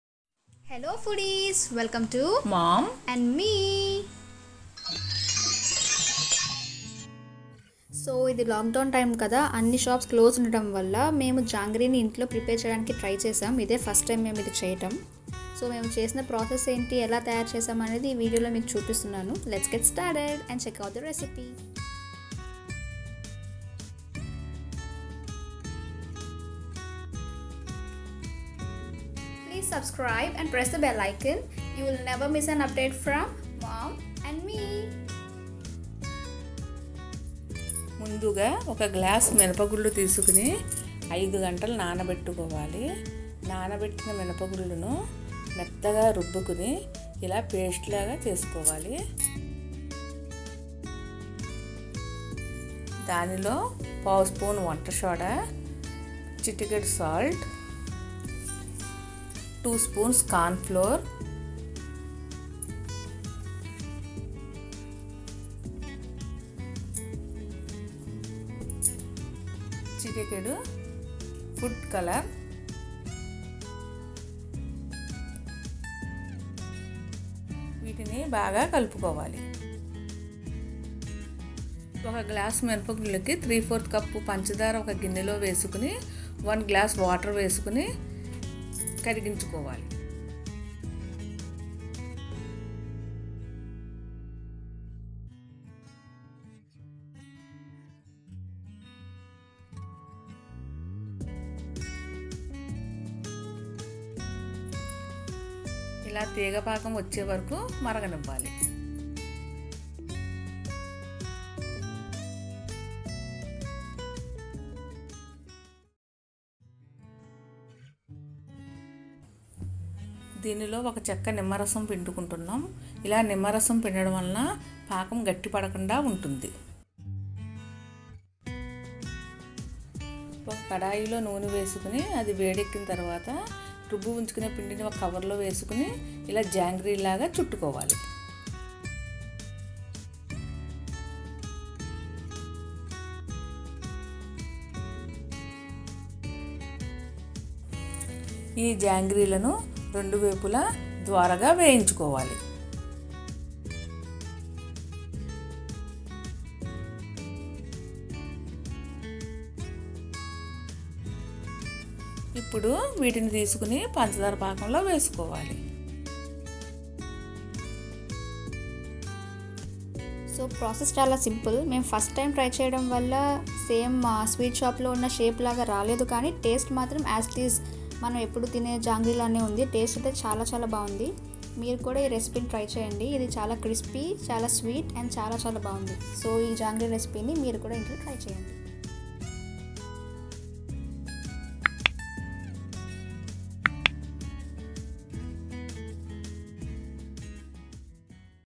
Home made jangri | Tried making jangri for the first time | Lock down cooking recipie
Hi Guys,

Try out the yummy jalebi at your home and let us know how did you like it.